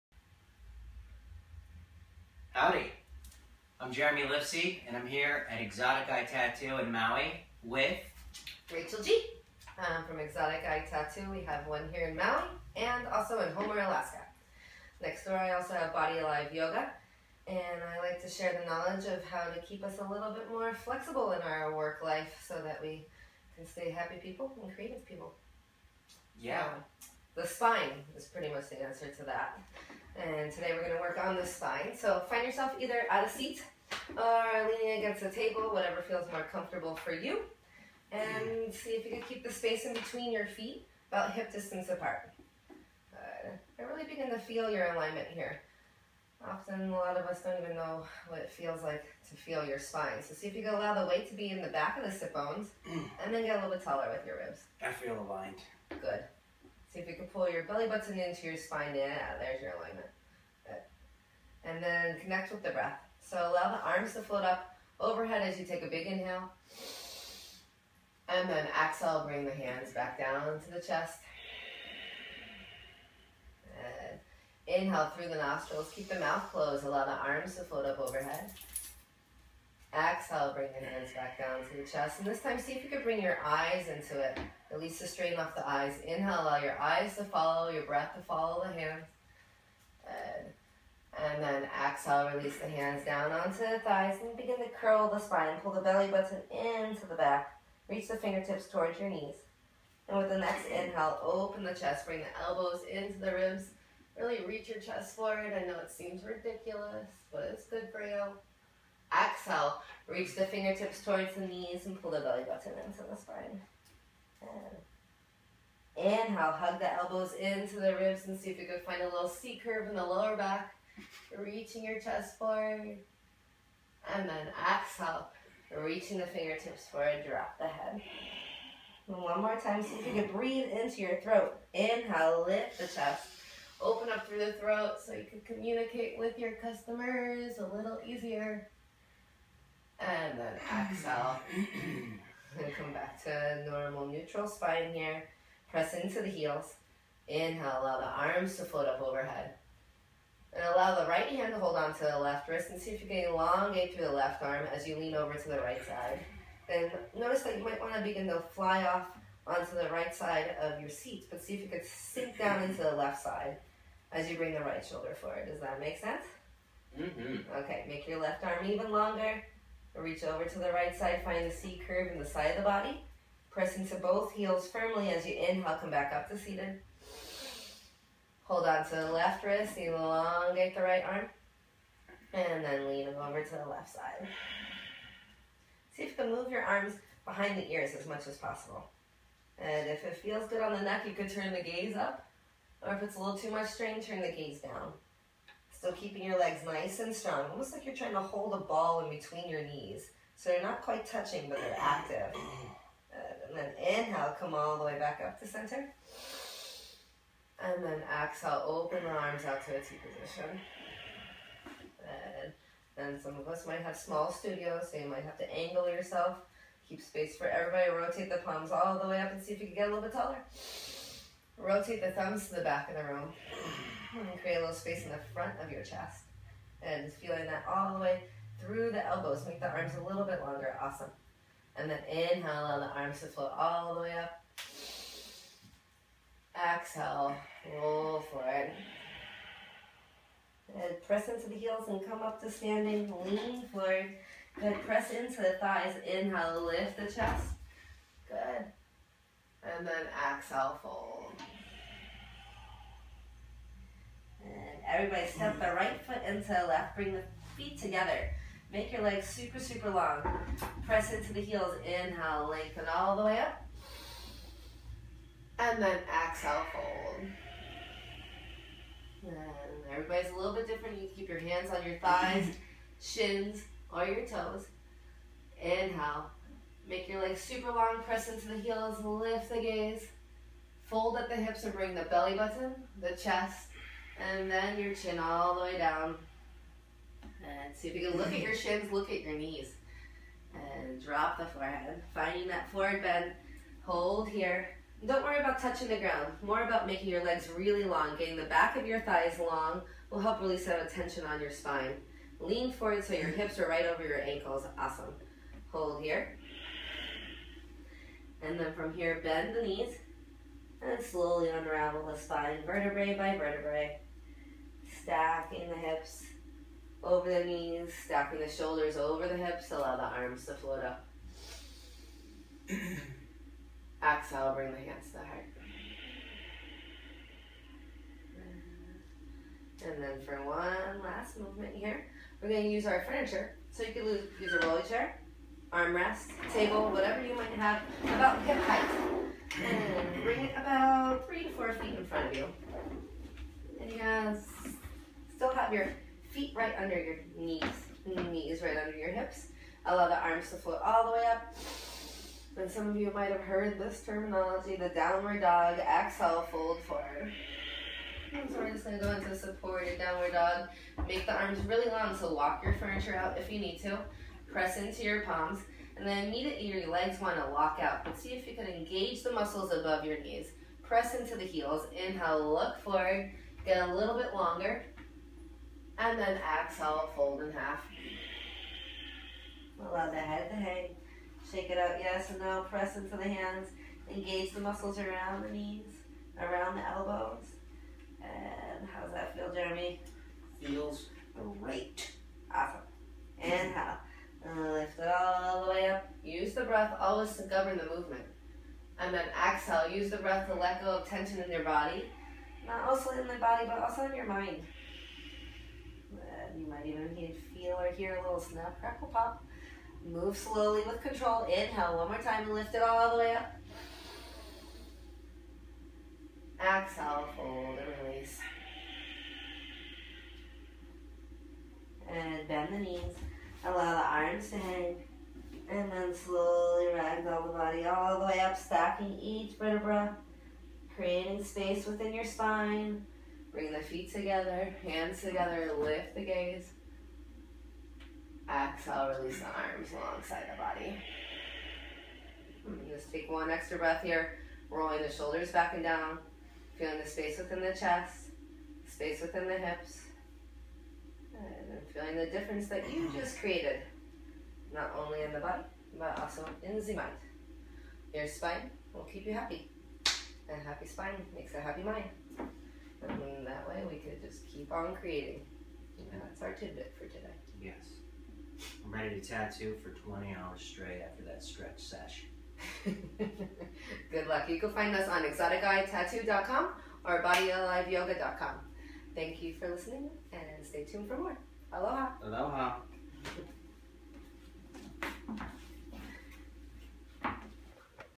Yoga for Tattoo Artists Back Stretches
Want some easy to do breath exercises and stretches to help you loosen up your back, increase your creativity and deepen your concentration? Check out this video that you can do anywhere, even at your desk!

For more information about RachelG and Jeremy Lifsey of Exotic Eye Tattoo check out: exoticeyetattoo.com